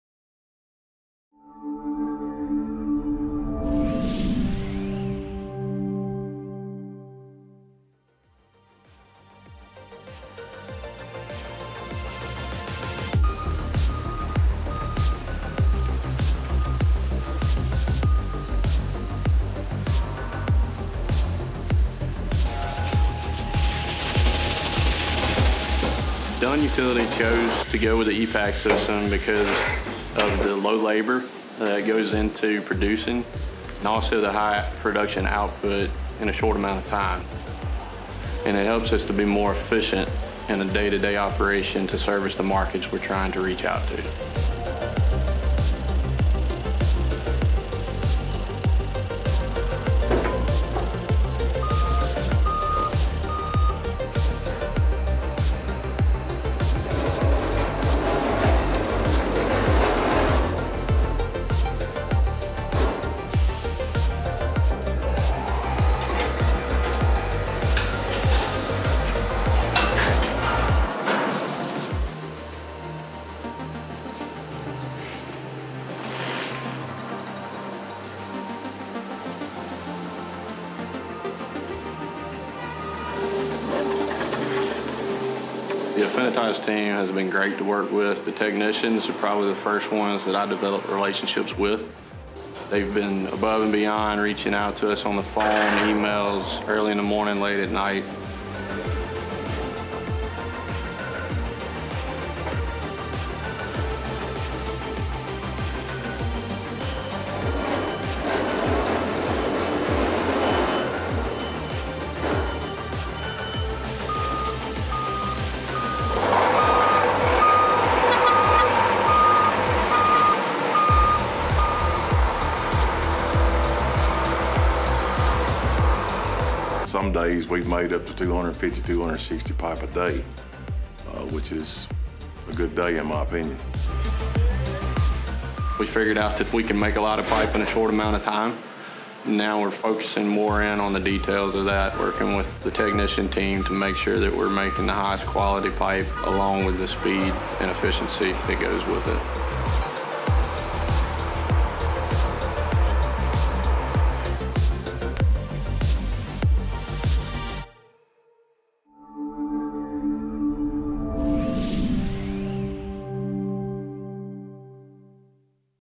HawkeyePedershaab ePak Packerhead at Dunn Utility
Afinitas recently installed a HawkeyePedershaab ePak Packerhead concrete pipe machine at Dunn Utility LLC in  New Albany, Mississippi. Dunn is pleased with the results and with the service they received from Afinitas. The video shows the machine in operation including its unique features like ePak's rollerhead drive that generates more power than conventional drive systems. The ePak includes precision production controls that help ensure quality and consistency in concrete pipe. Its main drive, toptable and bellpacker are cantilevered to continually bear and smoothly roll on the ePak's unique main frame resulting in less sharing and vibration, minimizing wear on components. Dunn chose the ePak for its extremely fast cycle times, superior control for producing excellent pipe and the operational savings it provides.